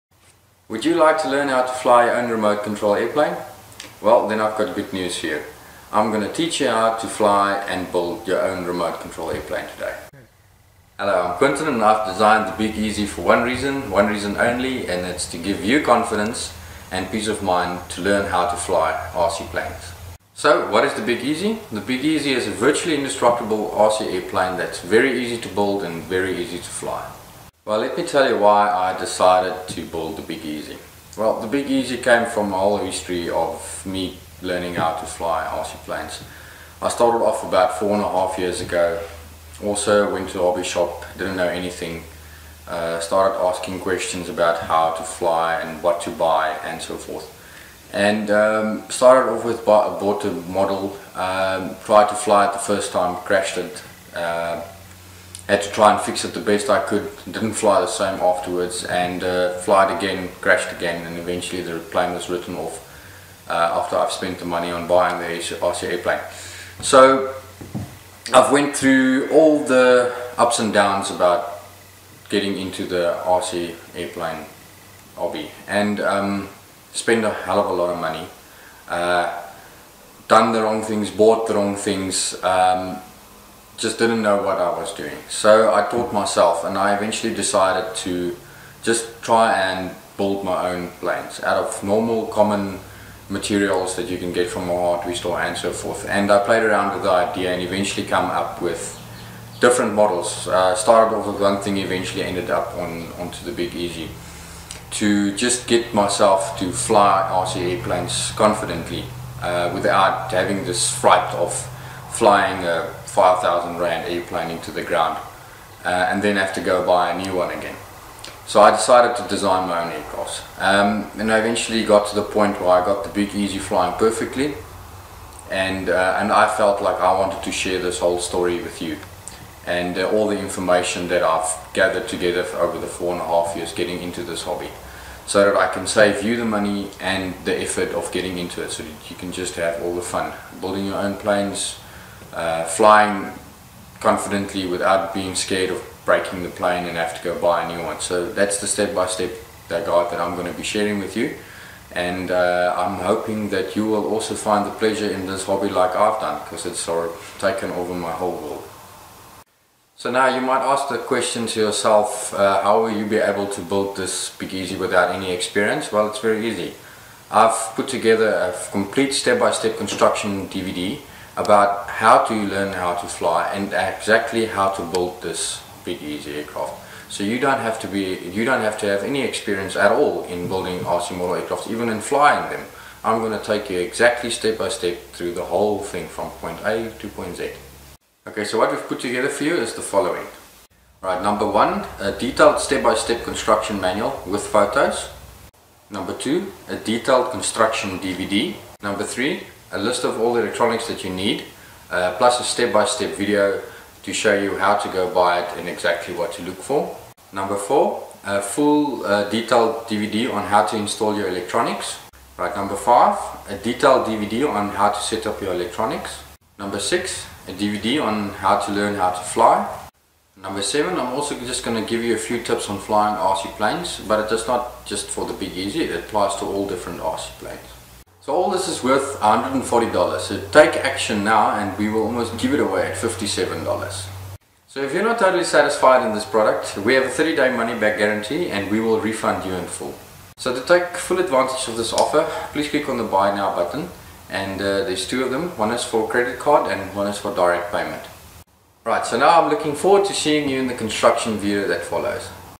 How To Build A Remote Controlled Plane
A Step By Step Guide On How To Build & Fly Your Own RC Plane